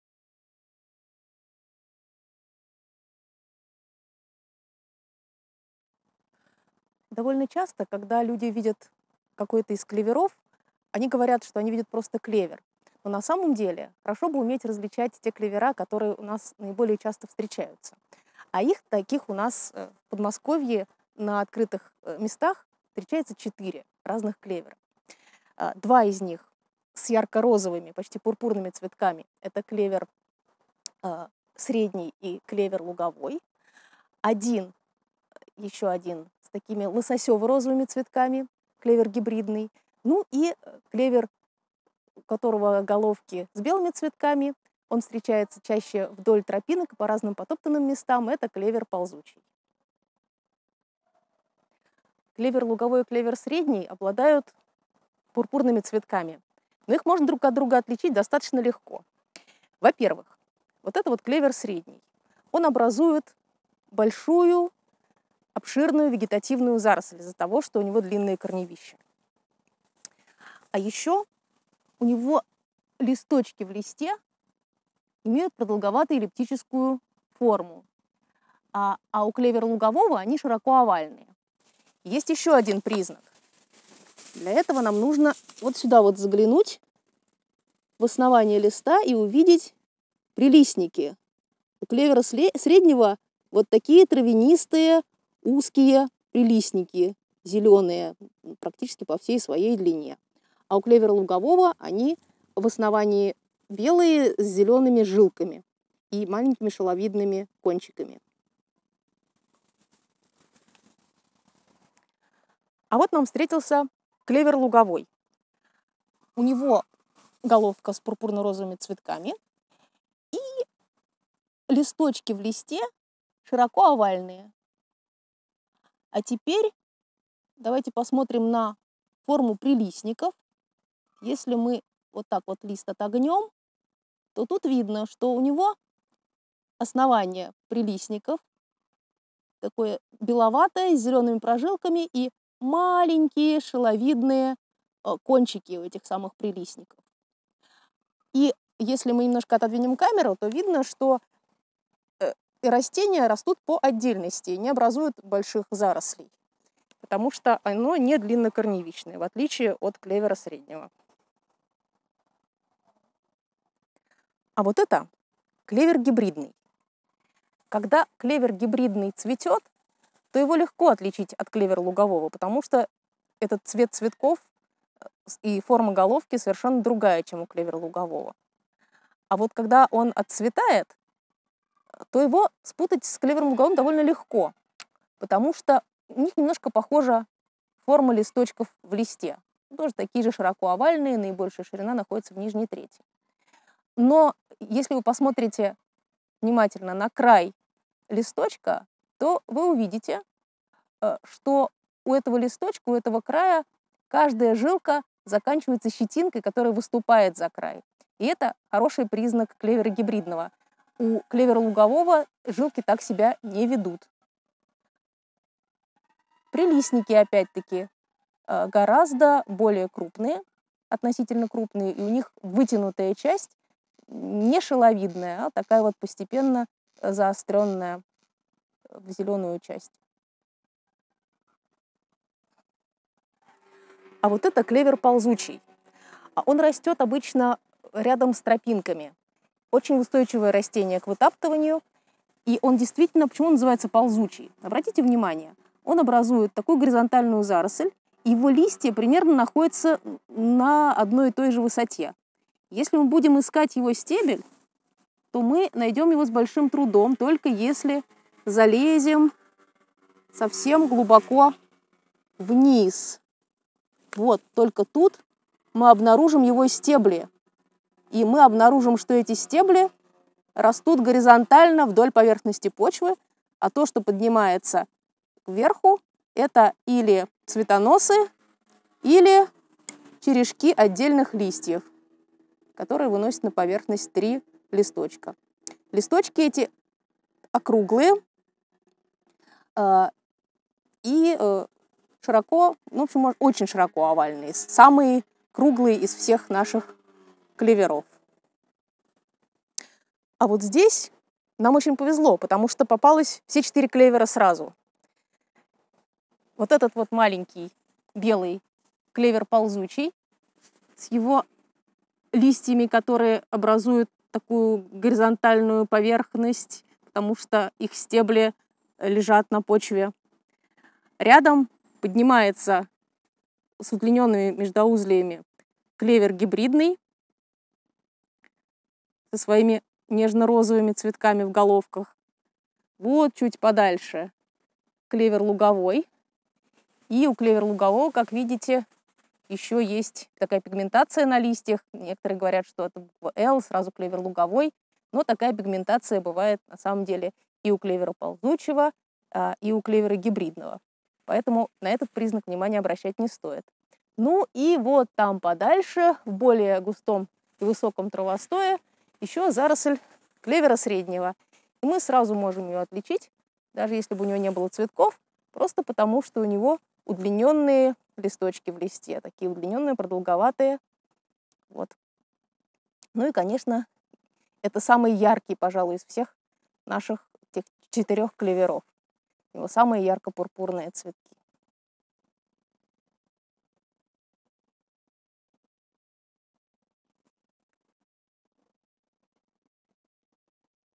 Клевер средний, К. луговой, К. гибридный, К. ползучий.
Клевер средний (Trifolium medium), Клевер луговой (Trifolium pratense), Клевер гибридный (Trifolium hibridum), Клевер ползучий (Trifolium repens) - наиболее часто встречающиеся в Подмосковье виды клевера.